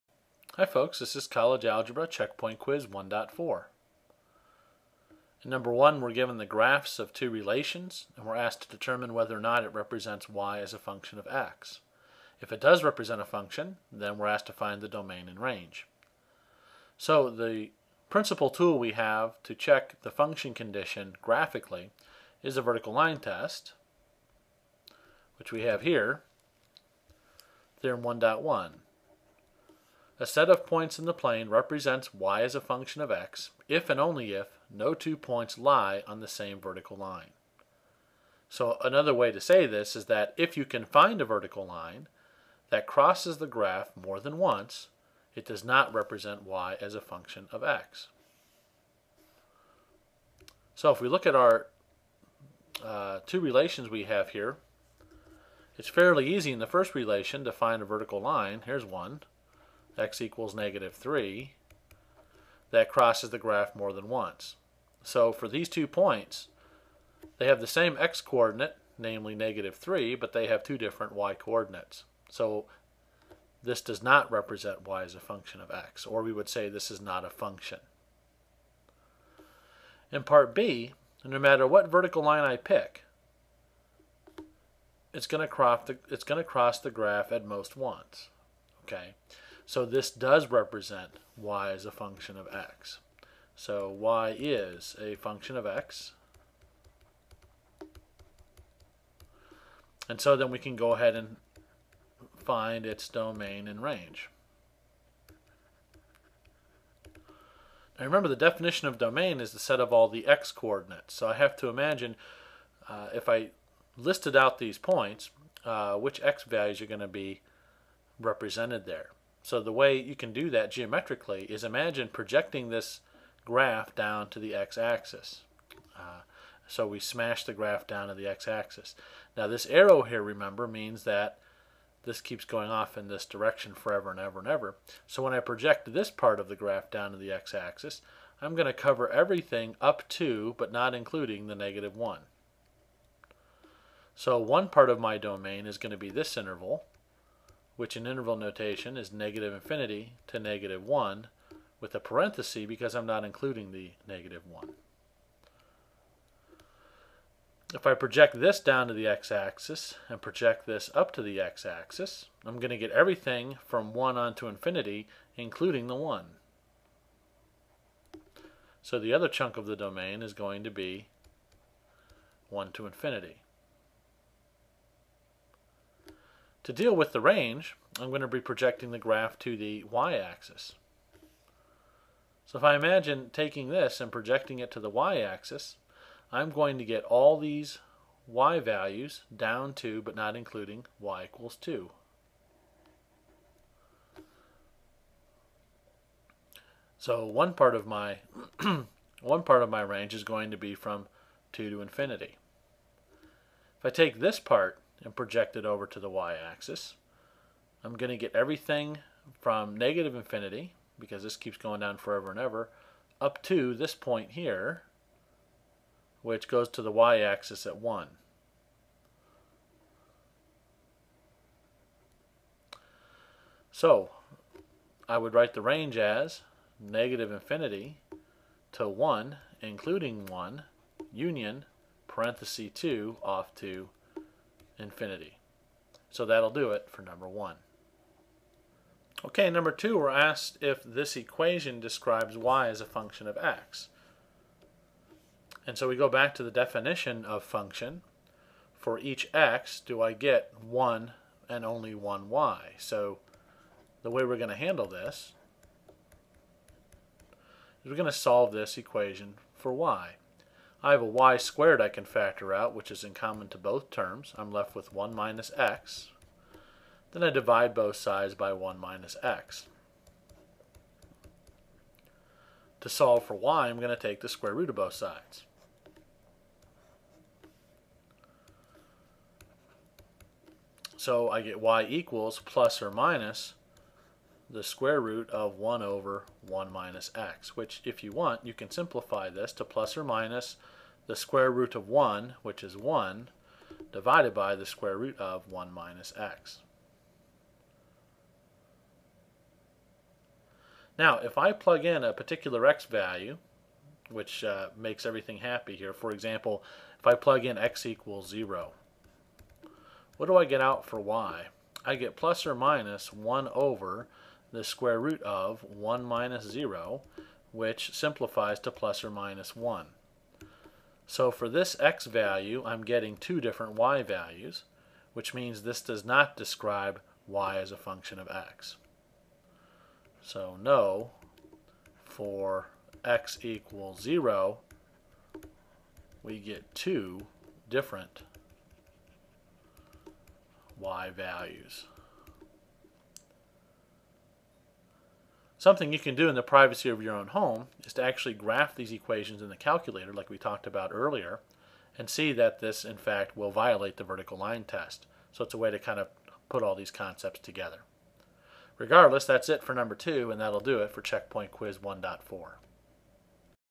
Stitz Zeager Checkpoint Quiz 1.3
We use the Vertical Line Test to determine which graphs represent y as a function of x, and find the domain and range of those which are.  We also determine if a given equation describes y as a function of x.